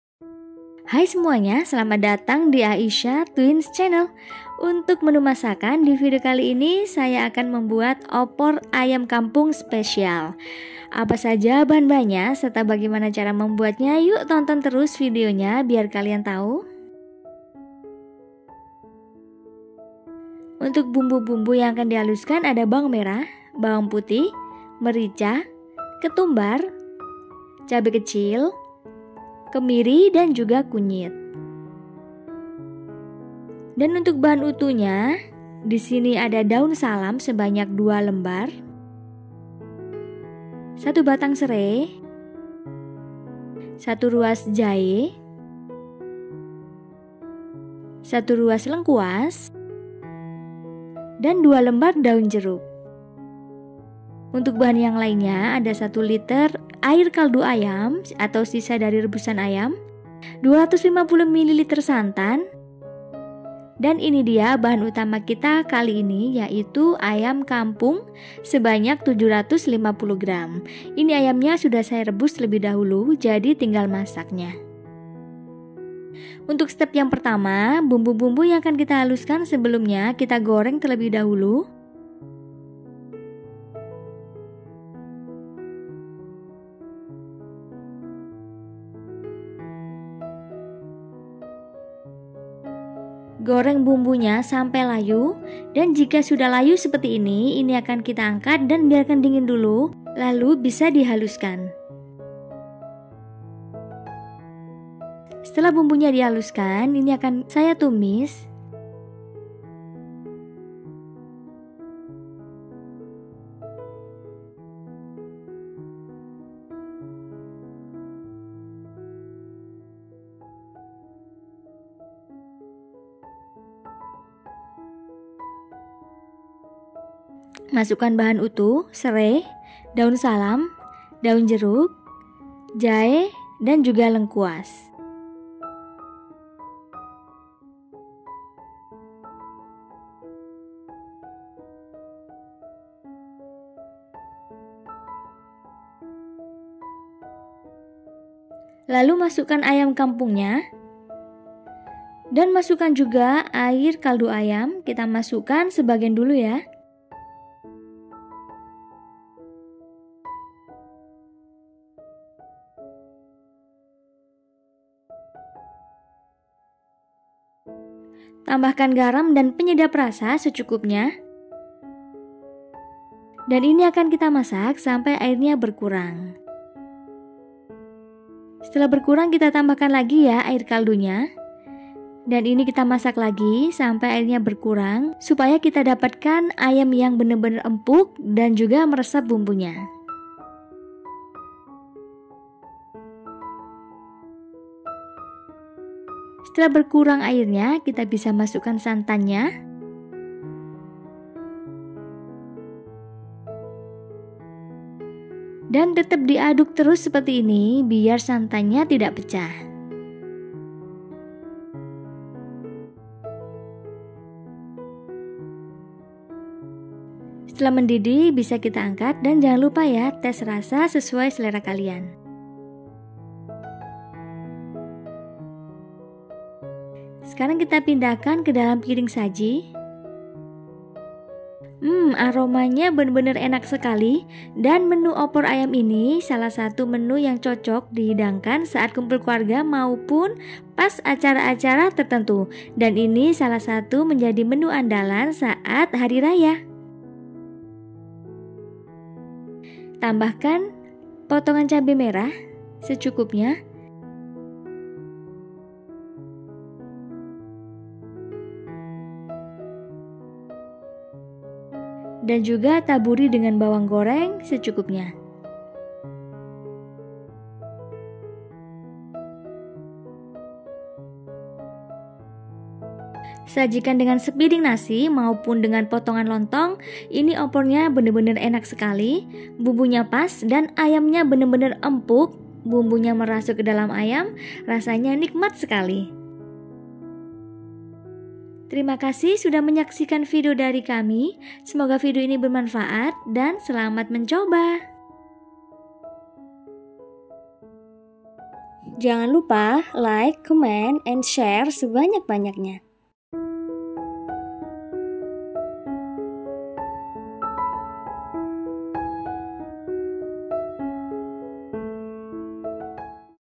Menu Hari Raya Super Enak | Opor Ayam Kampungn Spesial
halo resep masakan kali ini yaitu untuk menu Hari Raya, Opor Ayam Spesial
Happy Watching ...

OPOR AYAM KAMPUNG SPECIAL LEBARAN

BAHAN:
750gr ayam kampung sudah di rebus
1 liter air kaldu ayam
250 ml santan kental
Garam,penyedap rasa
2 lbr daun salam
2 lbr daun jeruk
 1 batang sereh
2 ruas jahe
2 ruas lengkuas
BUMBU HALUS
8 siung bawang merah
4 siung bawang putih
3 butir kemiri
5 buah cabe rawit
1 sdt ketumbar
1 sdt merica
2 cm kunyit